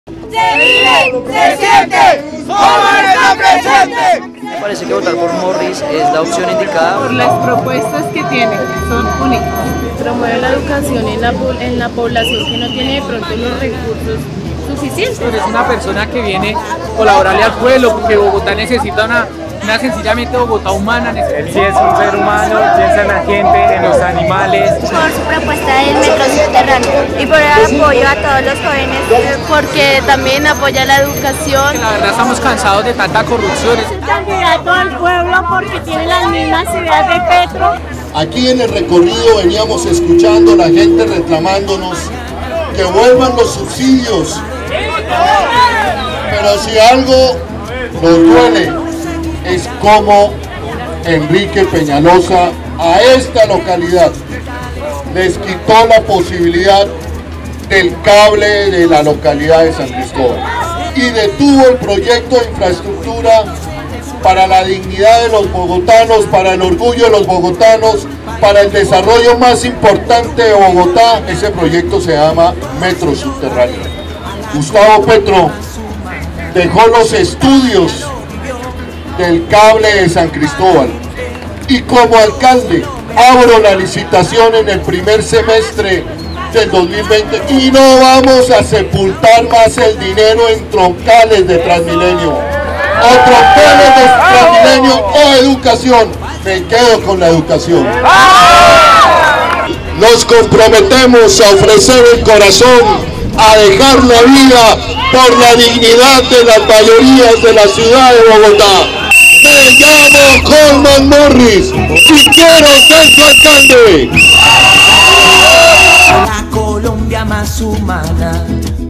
¿POR QUÉ VOTAR POR HOLLMAN MORRIS PARA LA LCALDÍA DE BOGOTÁ?
Tenemos el mejor Programa de Gobierno para ganar la Alcaldía de Bogotá y generar los cambios que la ciudadad necesita:

-Metro Subterráneo
-Reserva VDH como el Bosque Urbano más grande de América
-Universidad Pública y gratuita

NO MÁS PEÑALOSISMO.